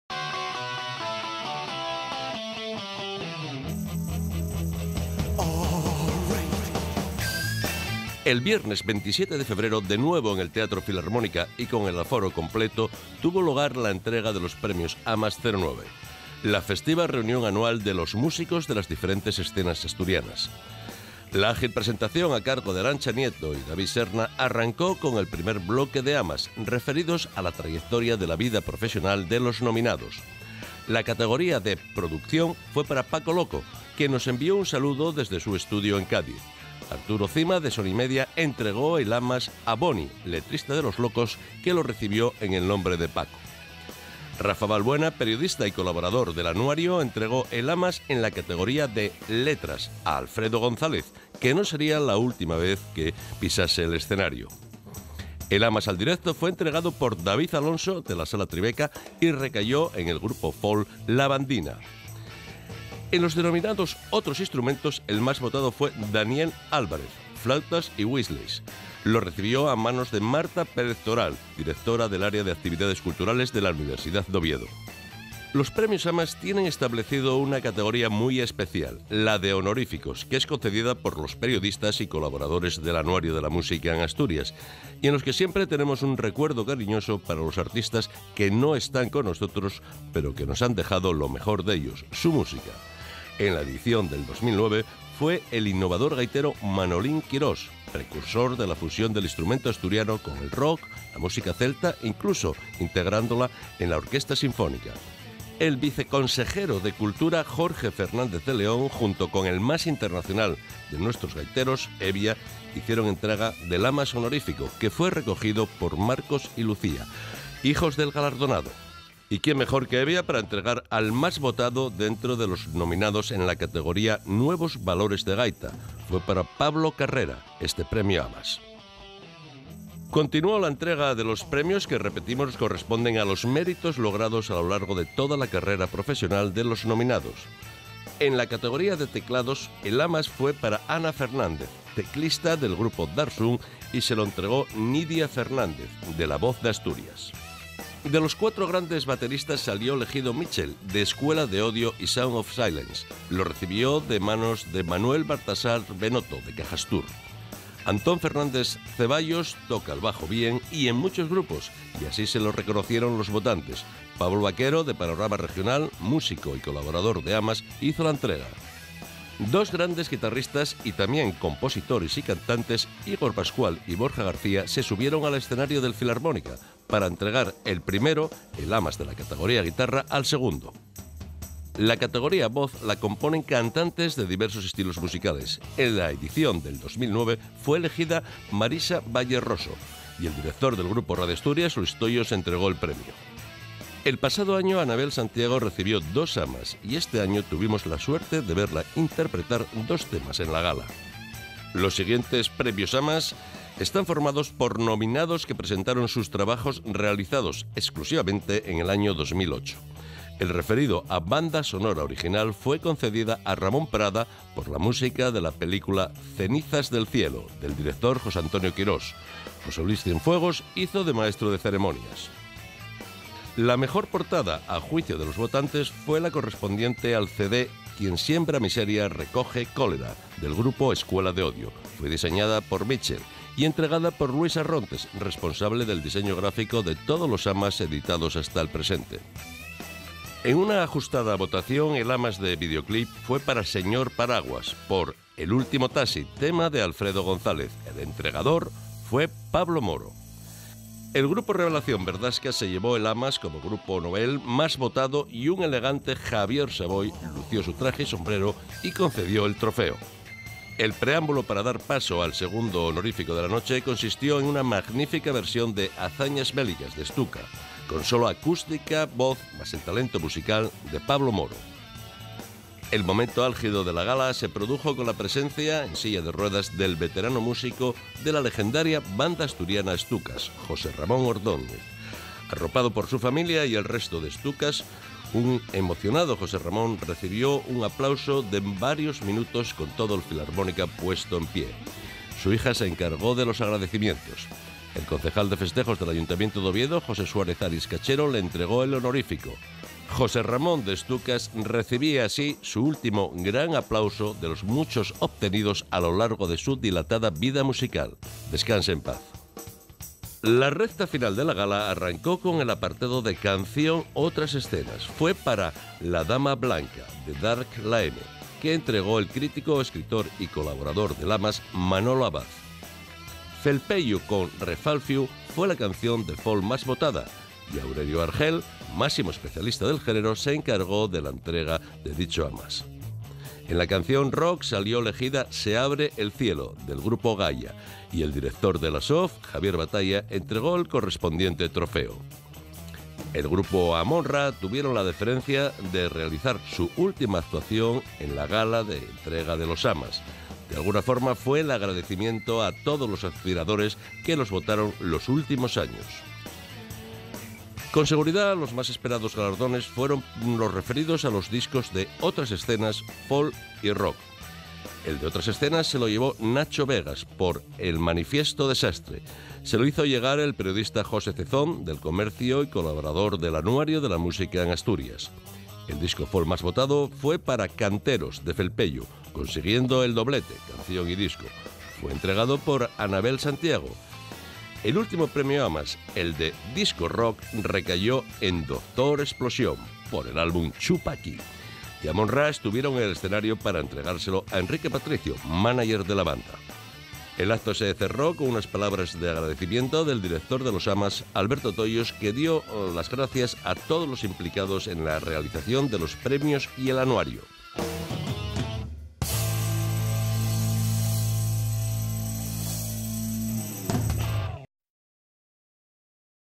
Premios AMAS 2009
Resumen de la gala de entrega de los Premios del Anuario de la Música en Asturias 2009.